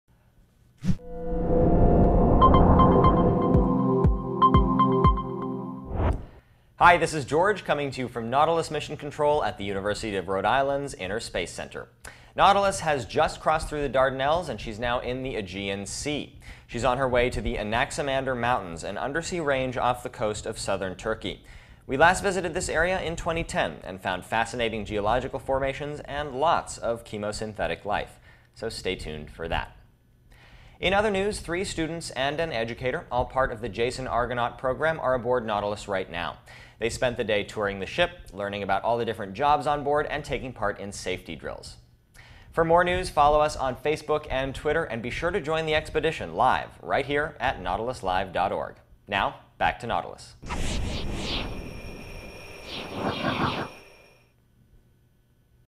Nautilus News Update - July 25, 2012
Stay up-to-date on the latest developments onboard E/V Nautilus. Nautilus is still in transit, on her way to the Anaximander Mountains, and she has some special guests aboard.